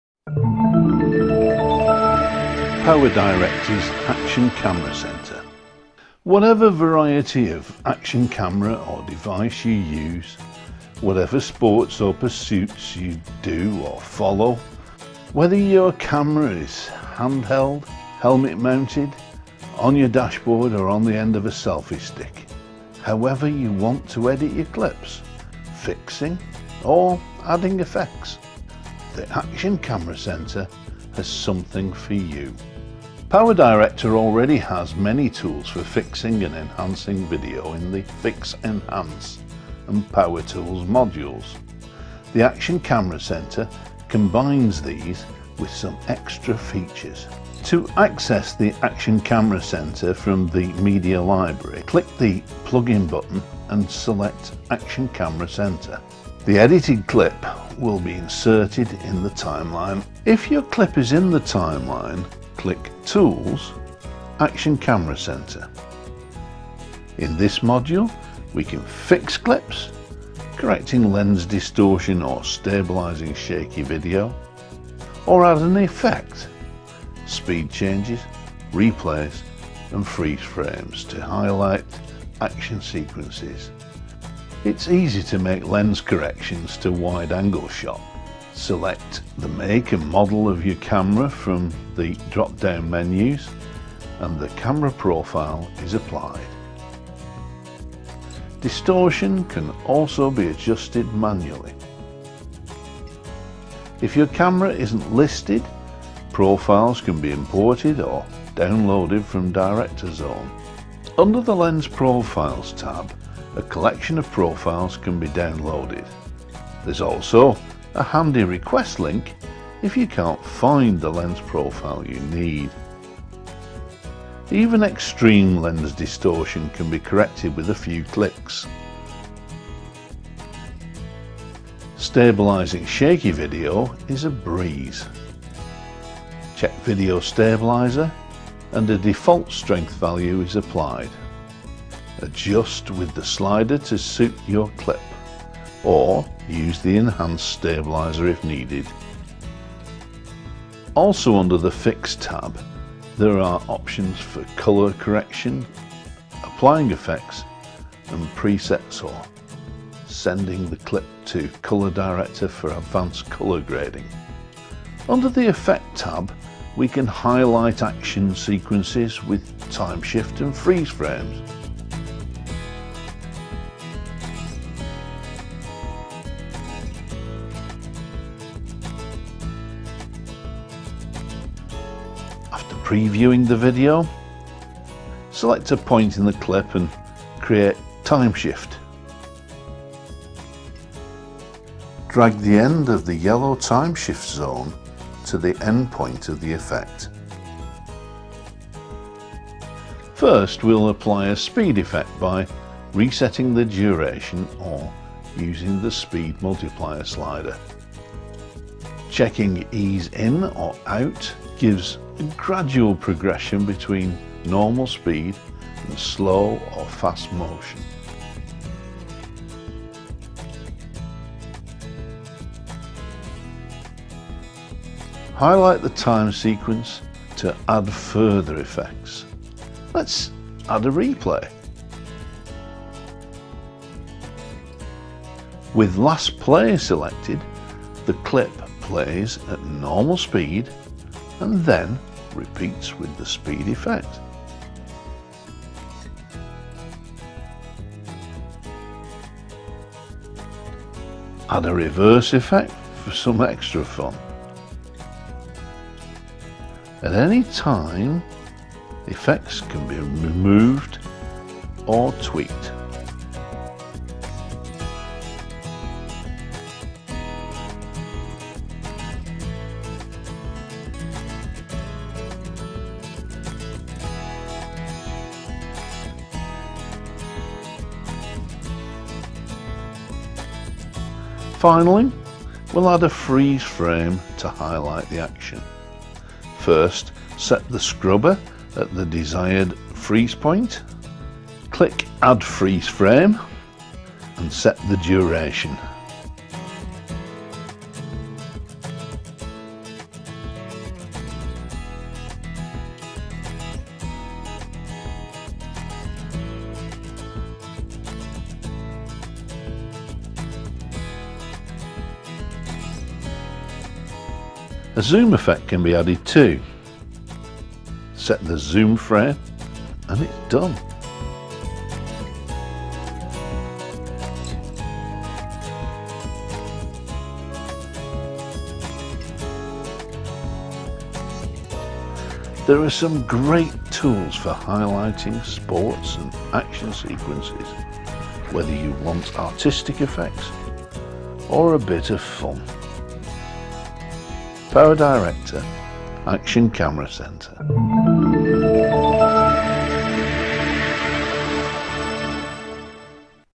威力導演教學影片 – 運動攝影工房
全新收錄專為運動影片剪輯愛好者量身打造的「運動攝影工房」，超強悍動感影音創作功能盡集於一處，輕鬆打造震撼影片。